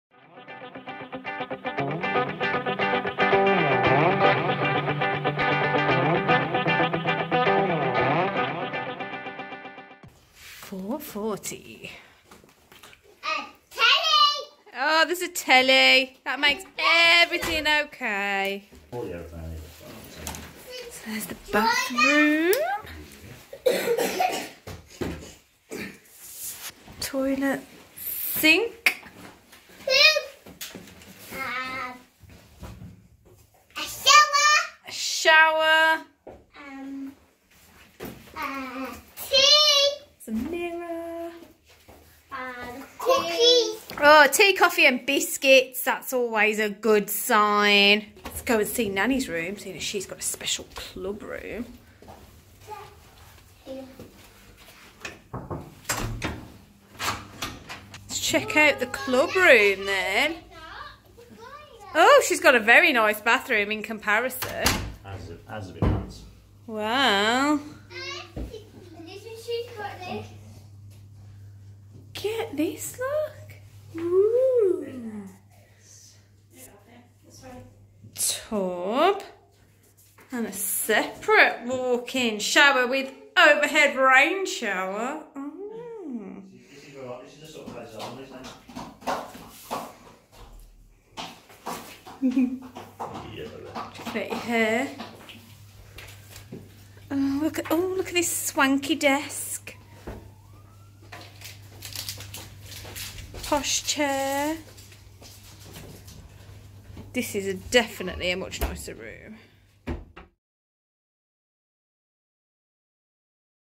Crown Plaza Gatwick Airport Family Room and Executive Room Tour - April 2019
Room tour of a Family Room and an Executive Room  at the Crown Plaza Gatwick Airport from April 2019.